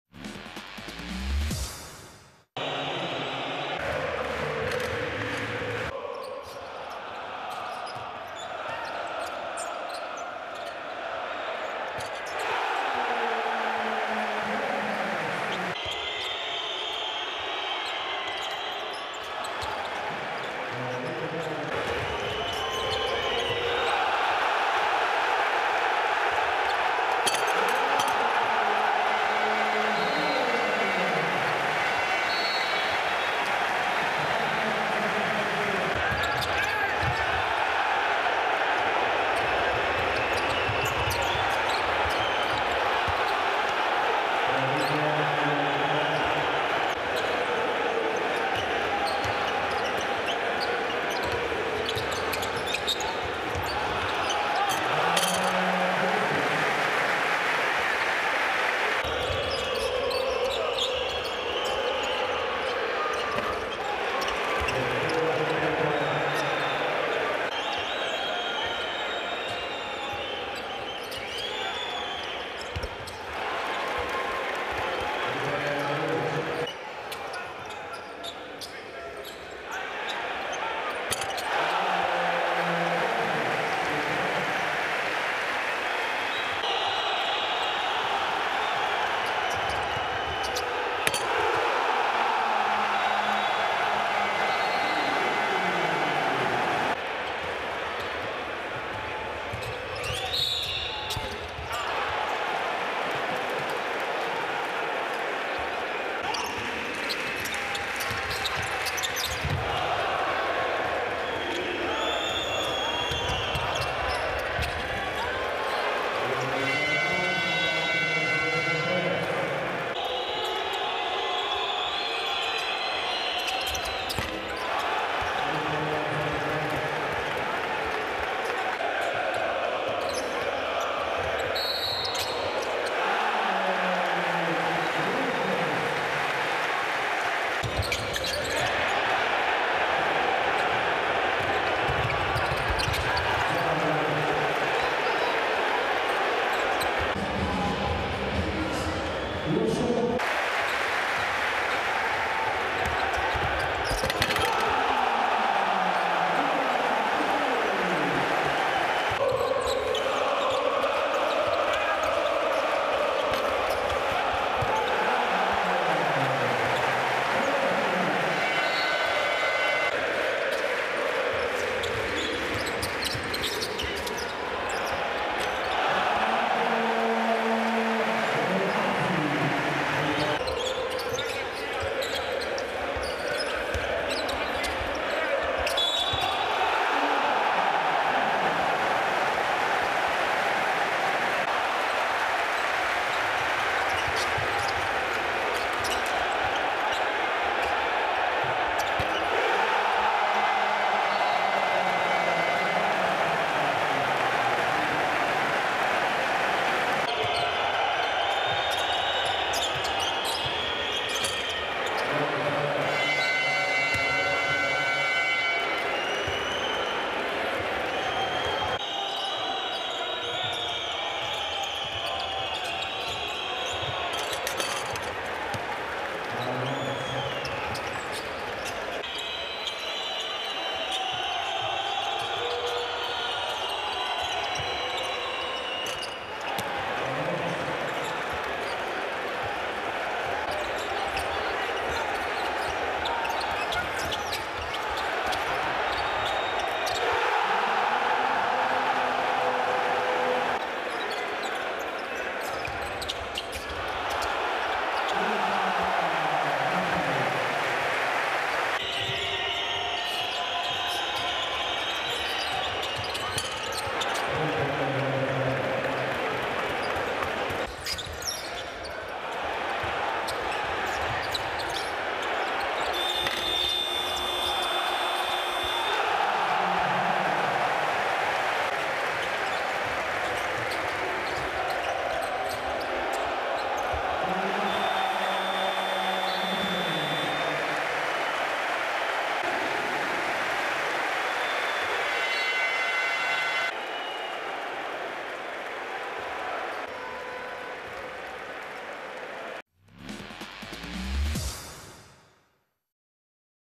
AdmiralBet ABA League 2021/22 highlights Playoffs: Crvena zvezda mts - Cedevita Olimpija (23.5.2022)
Take a look at the highlights of the 2021/22 season AdmiralBet ABA League Playoffs Semi-Finals game between Crvena zvezda mts and Cedevita Olimpija in Aleksandar Nikolić Sports Hall, Belgrade.